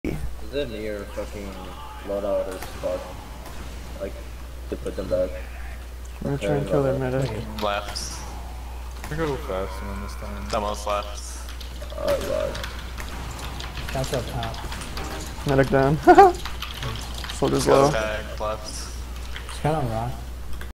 Process Rocket Sync Med Pick
How to instantly kill the medic on process middle with some well timed rocket jumps and synchronized rockets -- Watch live